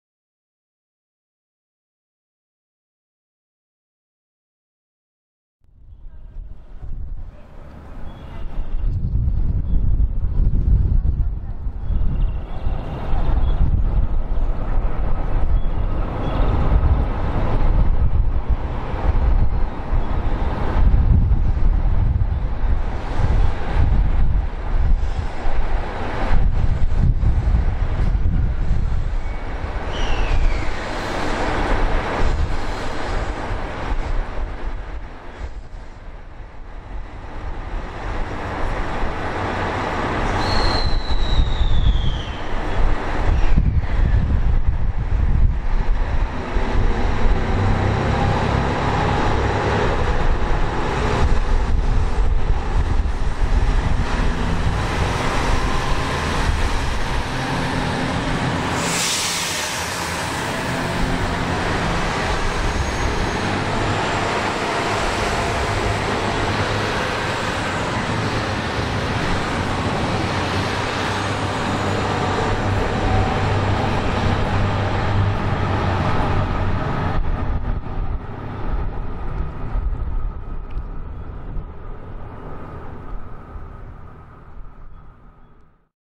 変態連結3ヶ所あり 特急南風7号+特急うずしお13号
瀬戸大橋線 児島駅に到着・発車する特急「南風7号+うずしお13号」を撮影しています。南風7号は2700系「きいろいアンパンマン列車」4両編成、うずしお13号は2700系3両編成で運転されていました。2700系には中間車両がないため、7両編成中に3ヶ所の変態連結ができていました。